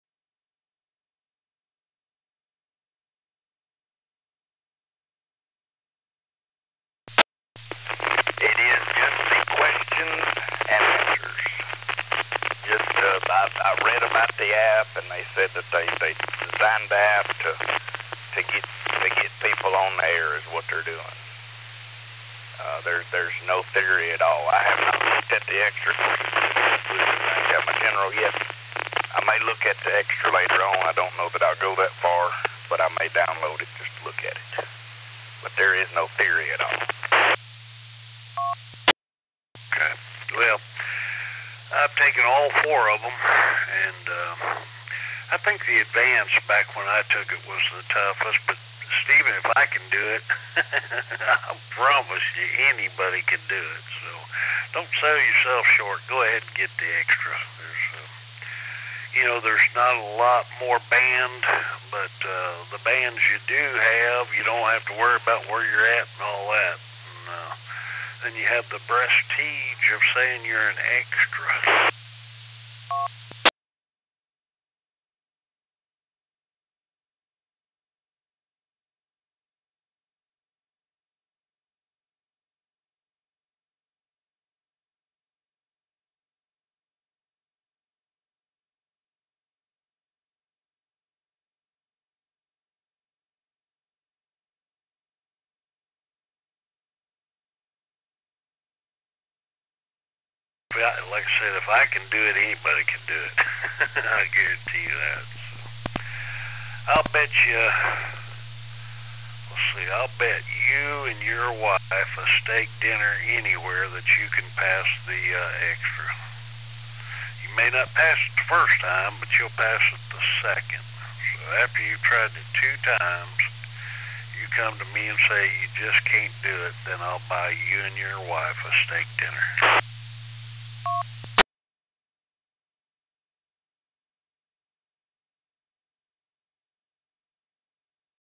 Scanner Plugin for SDR# Test
Using the $7 RTL dongle and SDR# (SDR Sharp) with the Frequency Manager / Scanner plugin.  The framerate of the capture software does not do this justice.  The scanner is uber-fast.